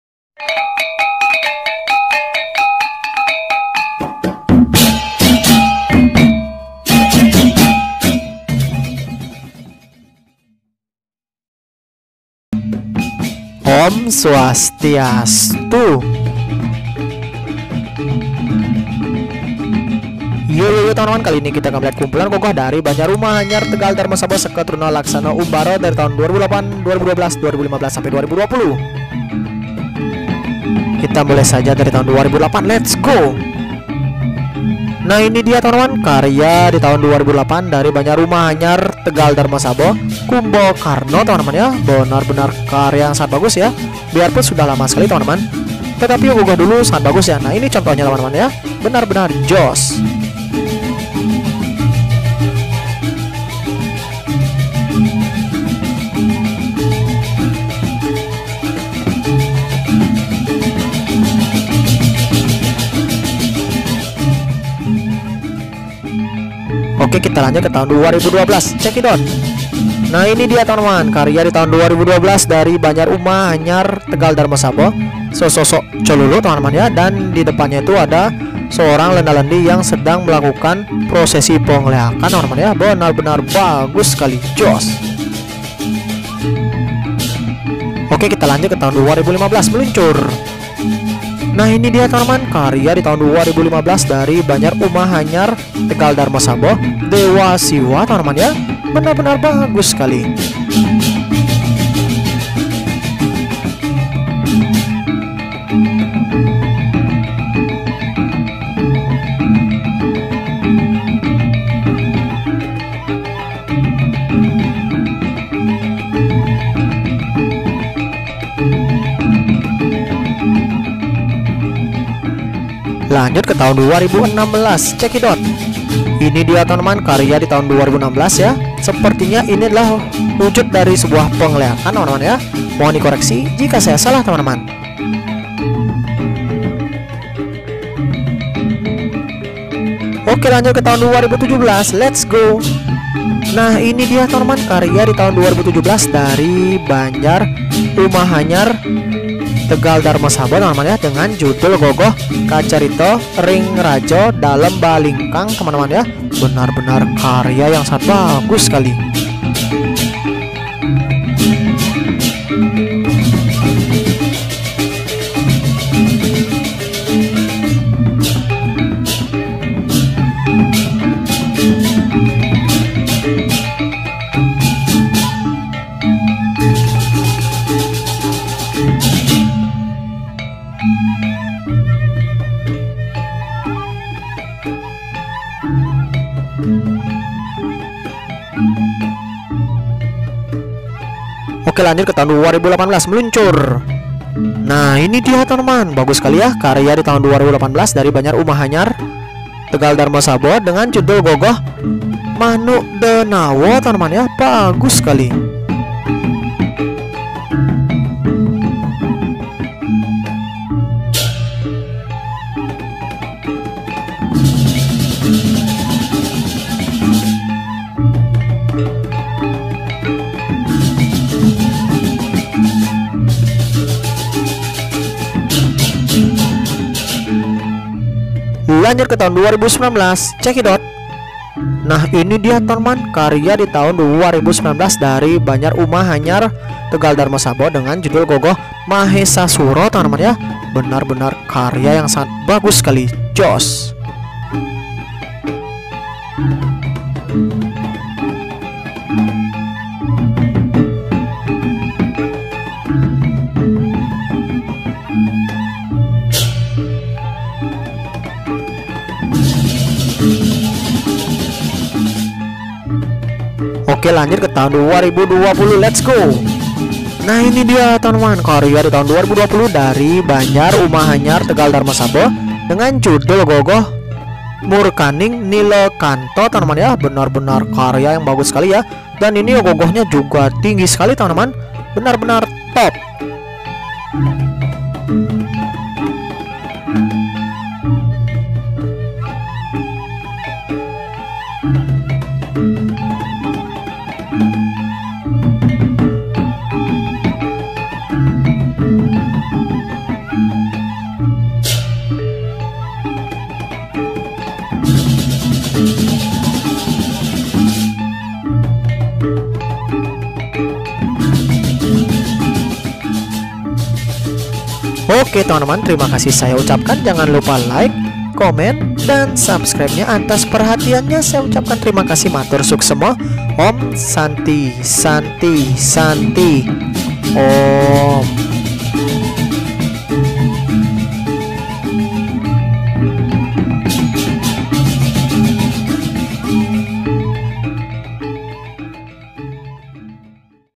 Keren.. Inilah Ogoh Ogoh Br. Umahanyar Darmasaba Dari Tahun Ke Tahun
Mohon ijinnya Br. umahanyar untuk repost dan didokumentasikan ogoh ogohnya di youtube 🙏